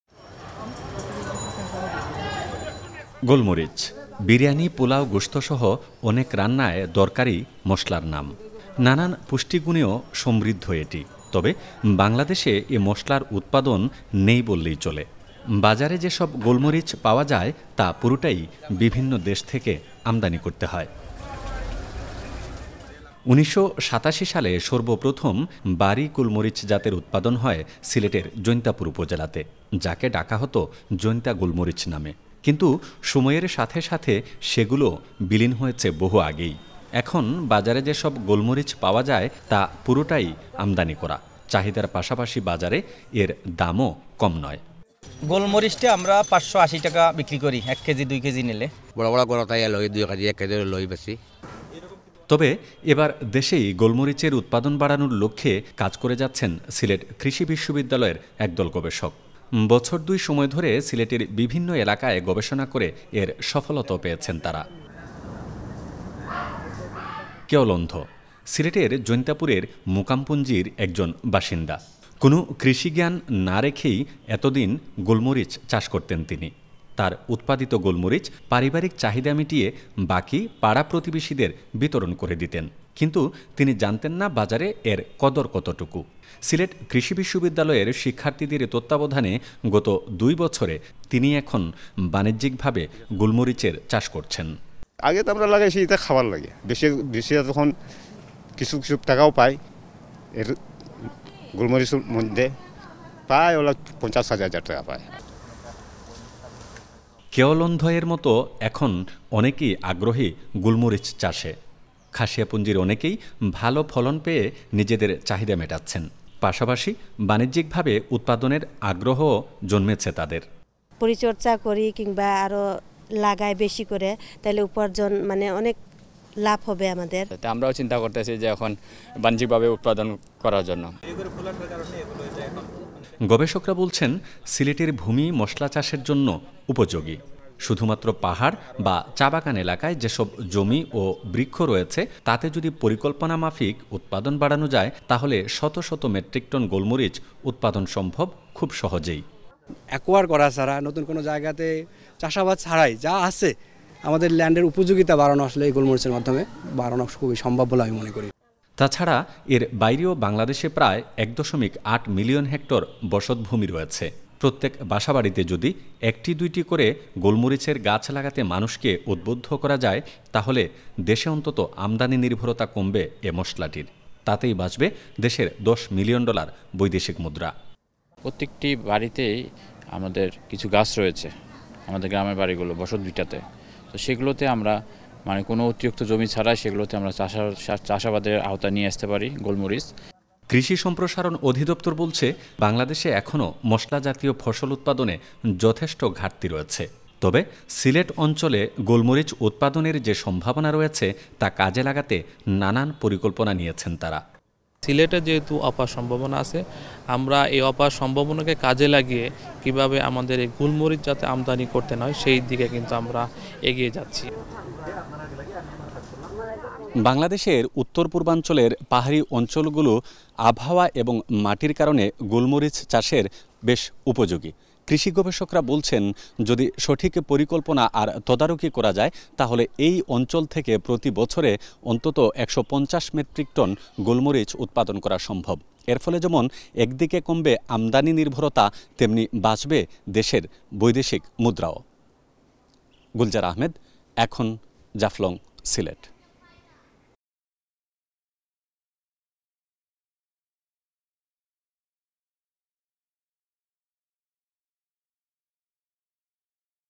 মসলা আমদানিতে বছরে খরচ ৩৭ কোটি ডলার! | Black Pepper | Ekhon TV
বাংলাদেশে মসলা জাতীয় ফসলের ঘাটতি অনেক। এজন্য চাহিদা মেটাতে বছরে দেশের আমদানি খরচ প্রায় ৩৭ কোটি ডলার। মসলা জাতীয় ফসল গোল মরিচ তার মধ্যে একটি। অথচ দেশেই এটি উৎপাদনের যথেষ্ট সম্ভাবনা রয়েছে। শুধু সঠিক পরিকল্পনা আর কৃষকদের সহযোগিতা দিলে দেশের চাহিদা মিটিয়ে ডলার বাঁচানো সম্ভব- জানান কৃষি গবেষকরা।

Our Office Address:
EKHON TV
City Park Lane
19, Hatkhola Road, Wari
Dhaka-1203
Bangladesh